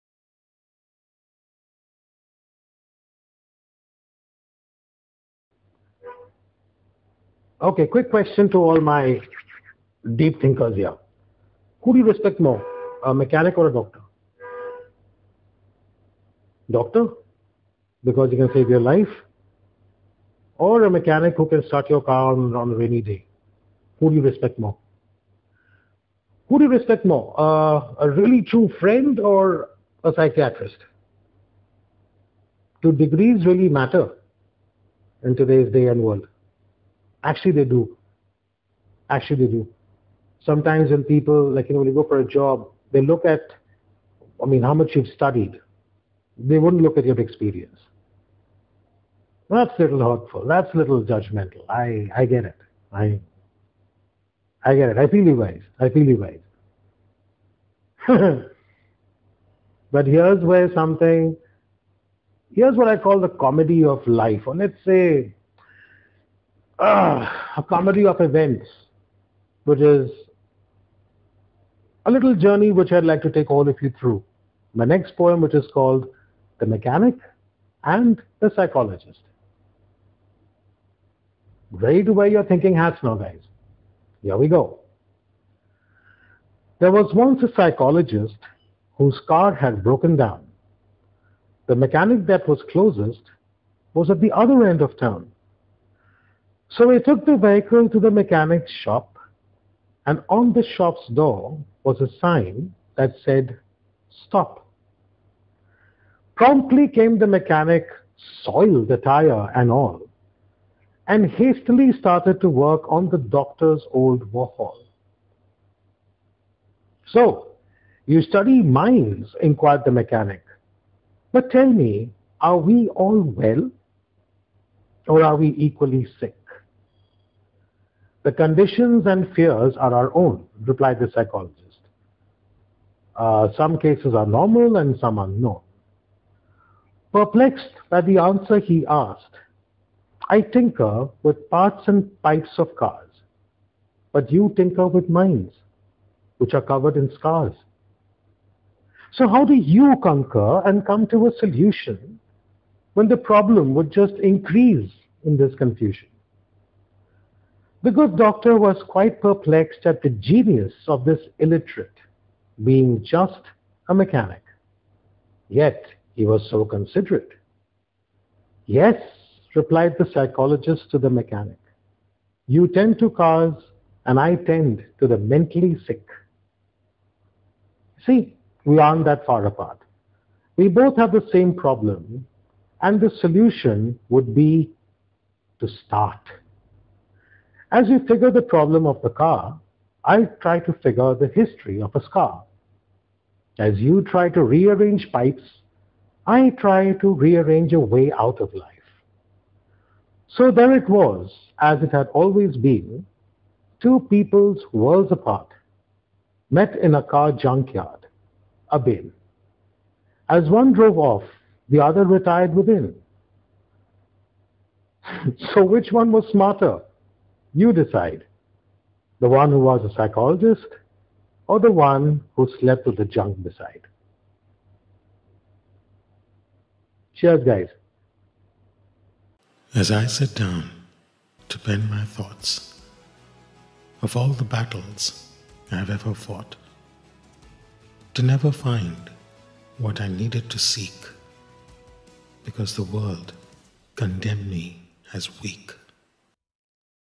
The Mechanic And The Psychologist
There is a marked difference between being educated and being literate, a stark difference between street smart and book smart, but ironically irrespective of what we know by way of books or by experience we tend to meet on similar and familiar grounds. Enjoy this short poem which hints at the familiarity of it all